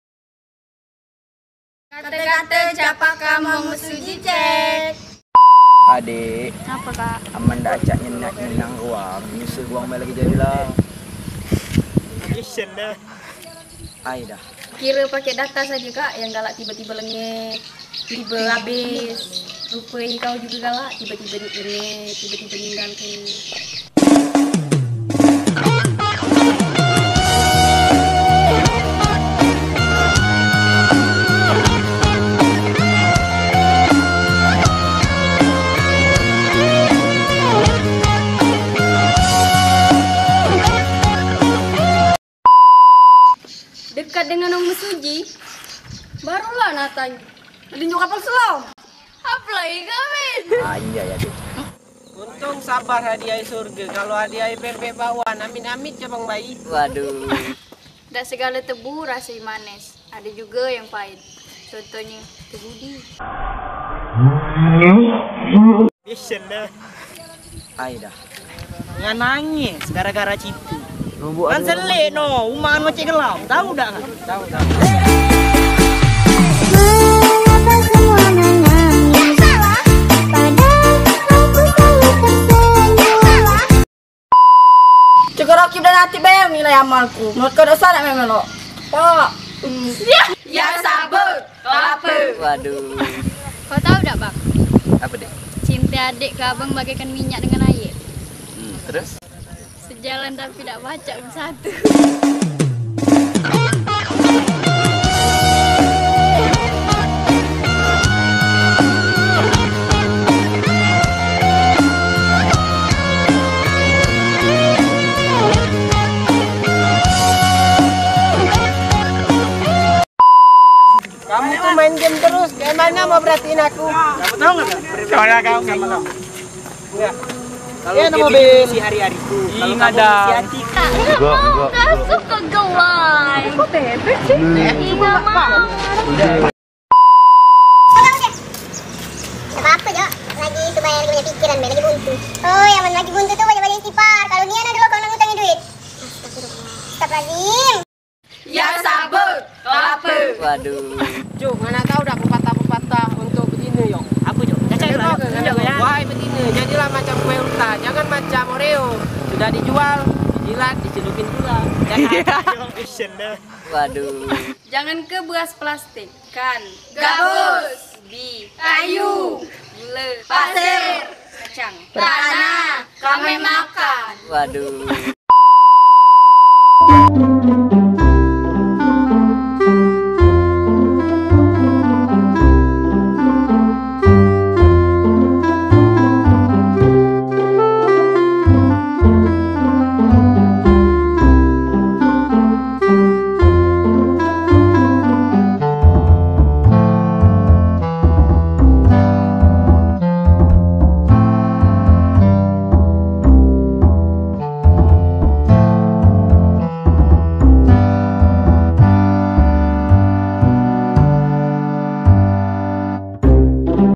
KELAKAR WANG TOBO ; Cak Pakam kaune 🤣💯
Kolaborasi dengan Siswa-Siswi SMK Tri Laga Utama
Eksplor budaya Mesuji